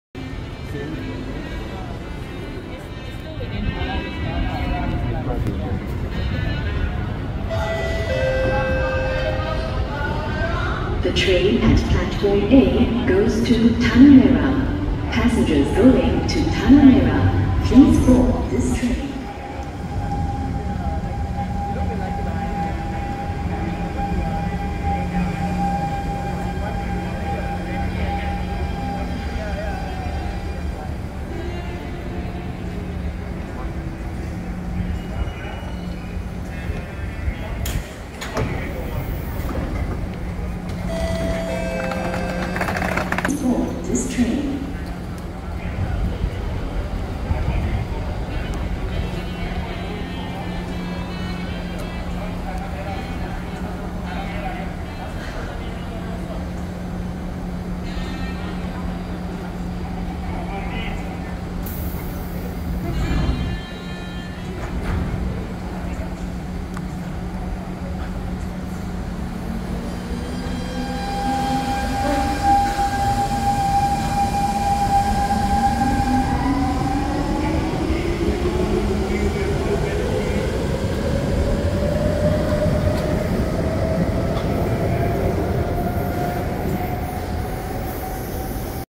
(1st C651 to wrongrail)[SMRT] C651 201/202 CGL at CG1/DT35 Expo
This C651 running as T102 on Changi Airport Line. Stopping mostly at platformB from CG1/DT35 Expo onwards. This for wrong rail.